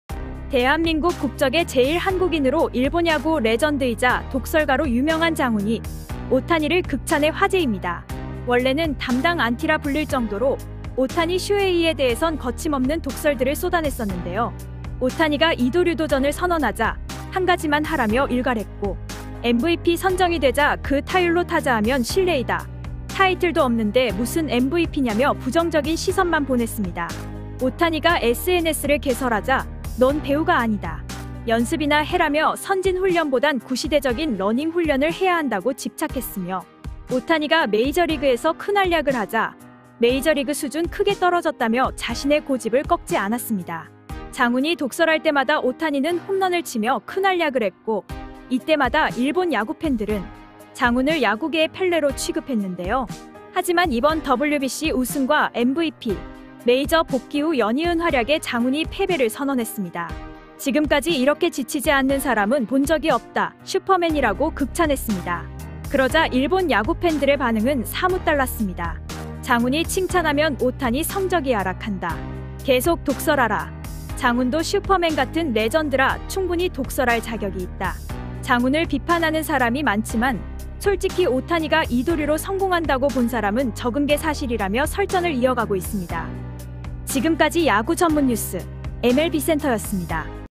오타니 담당 안티 장훈, 태도 변화 "오타니 지치지 않는 슈퍼맨" 극찬
일본 프로야구 레전드이자 재일 한국인 장훈 (하리모토 이사오)이 오타니 쇼헤이에 대해 독설이 아닌 극찬을 해 화제입니다